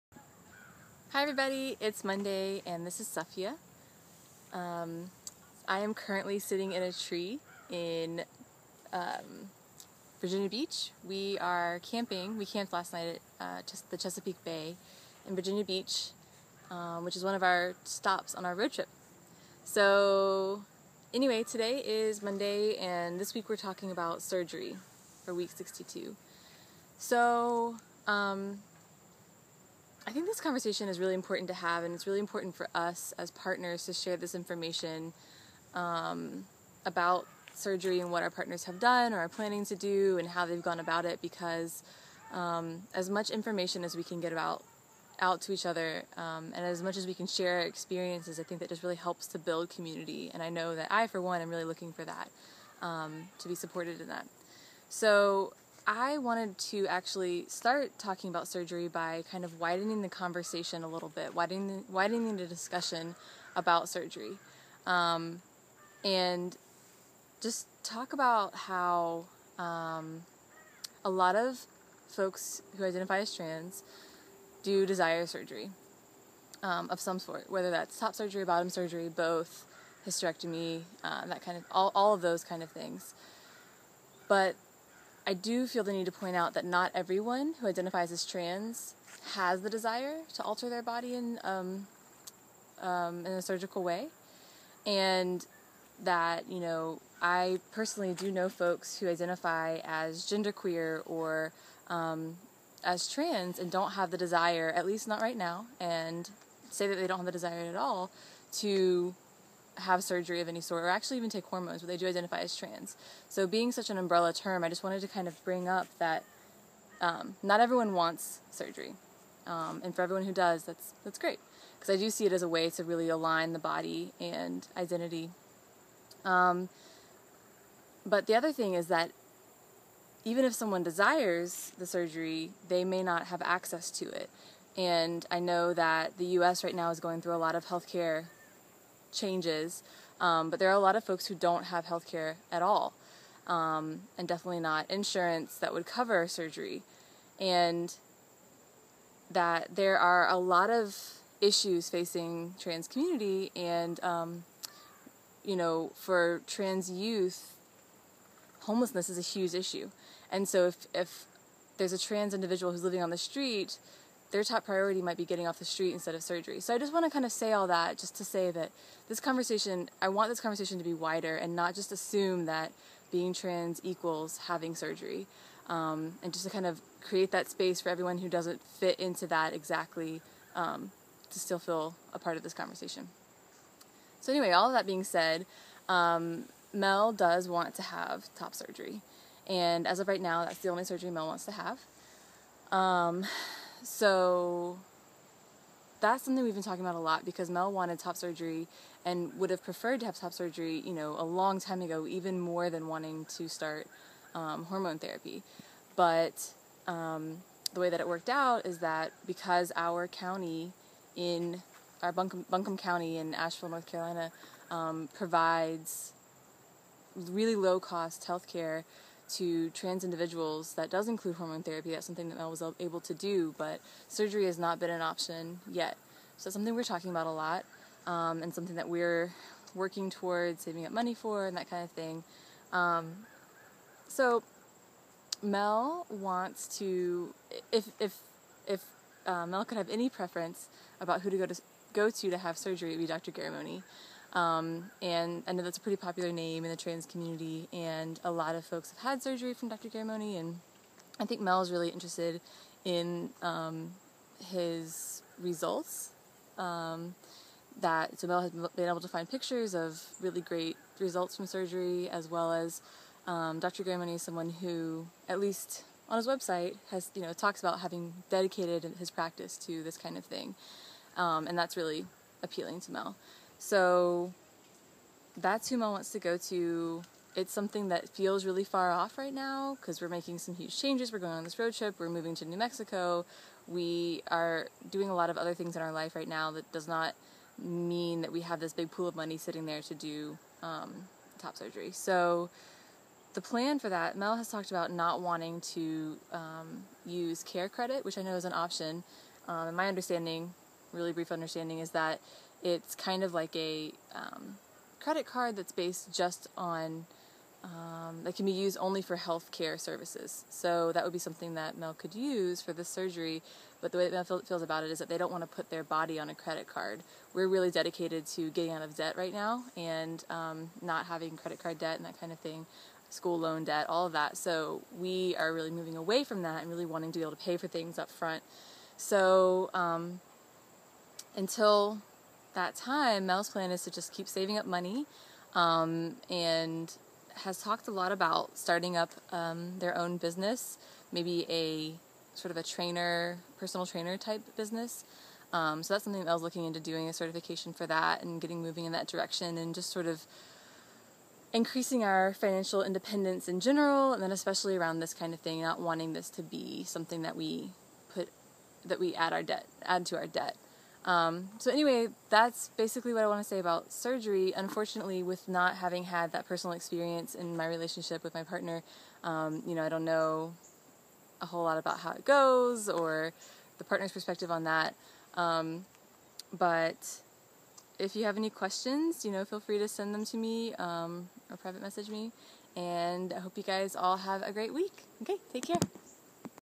W62: Surgery
Sitting in a tree taking about surgery :)